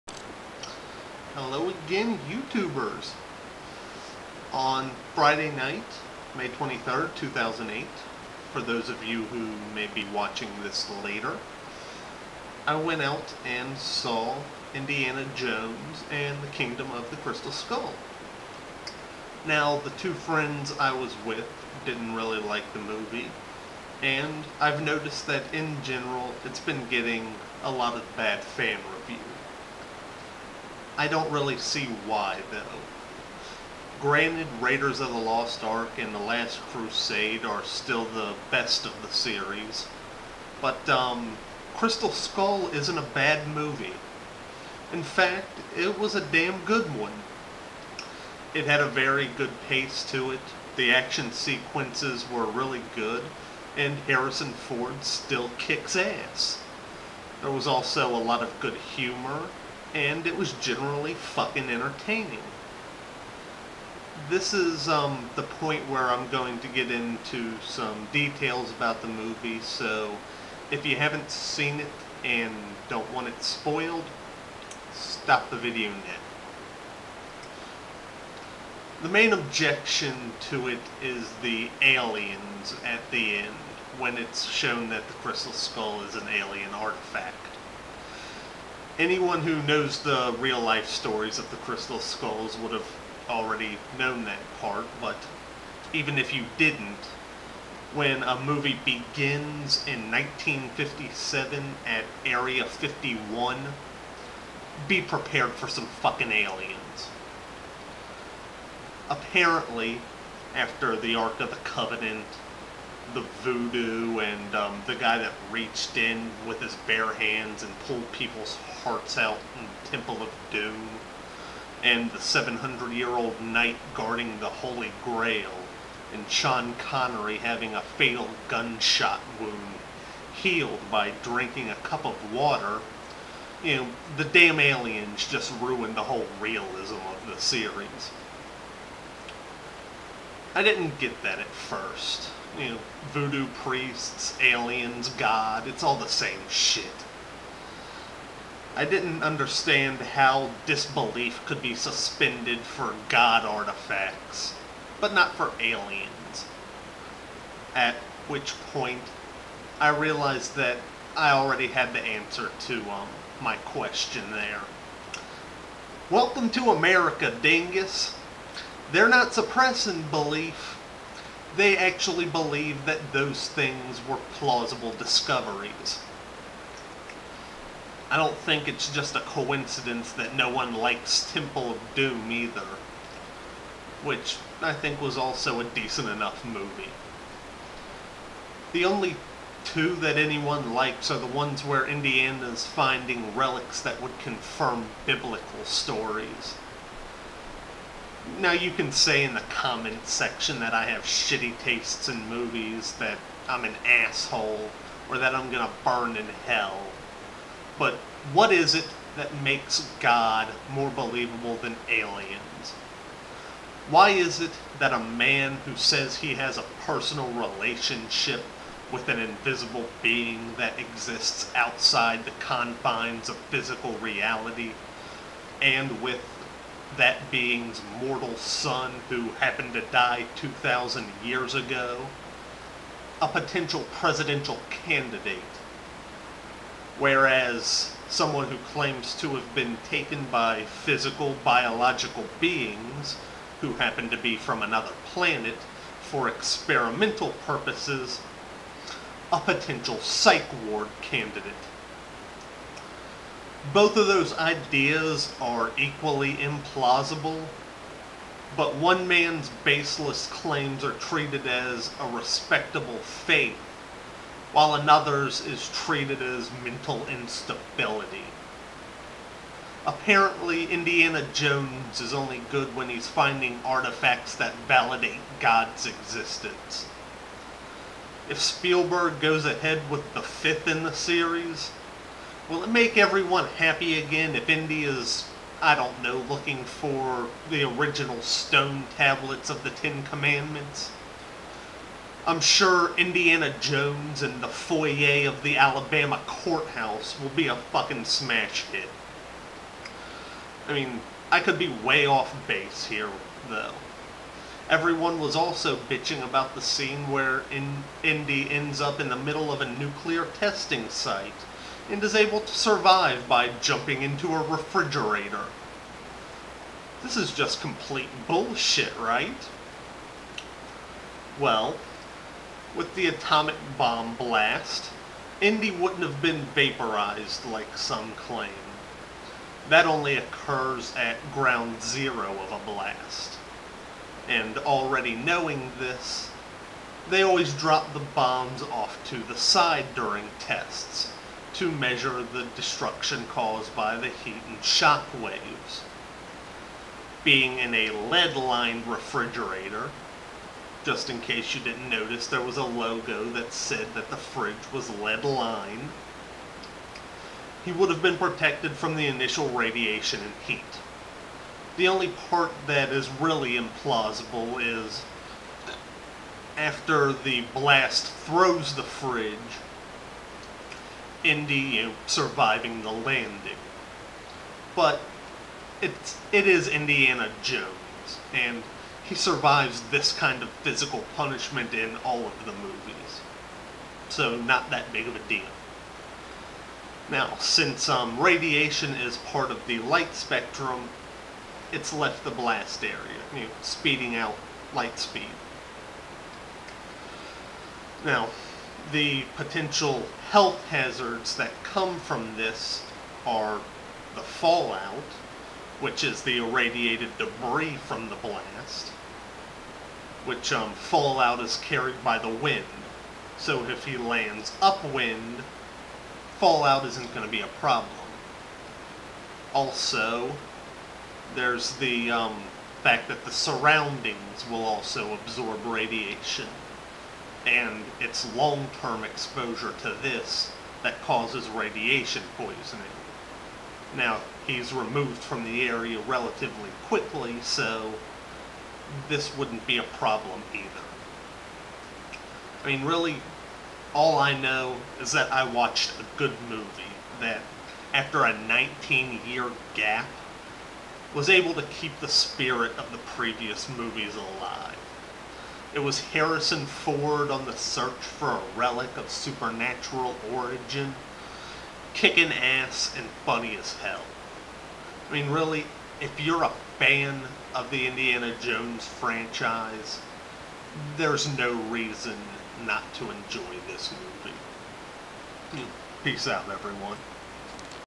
Indiana Jones and the Blatantly Obvious Double-Standard
The new Indiana Jones movie is a decent enough movie that is catching a lot of undeserved flak.

For my regular viewers, please watch the video to understand the basis of why I made this particular one.